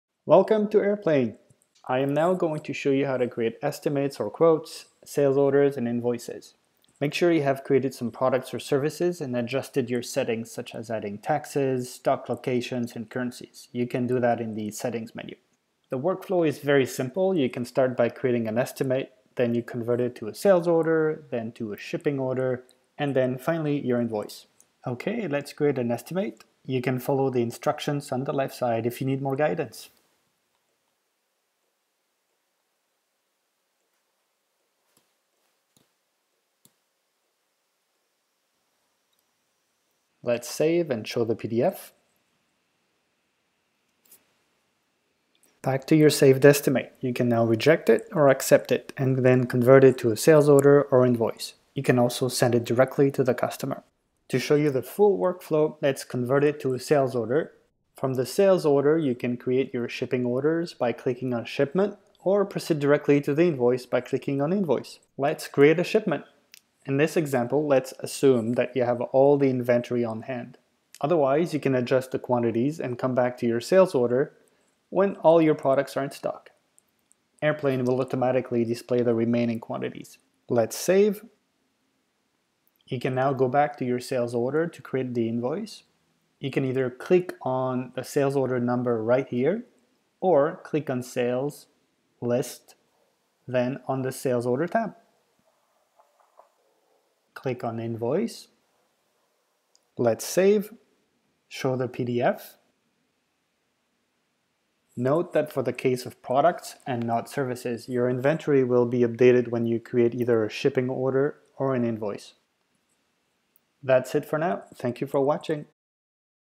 Manage Sales with erplain
This video will show you how to manage your sales more efficiently creating estimates, sales orders, shipping orders, and invoices.

Any questions?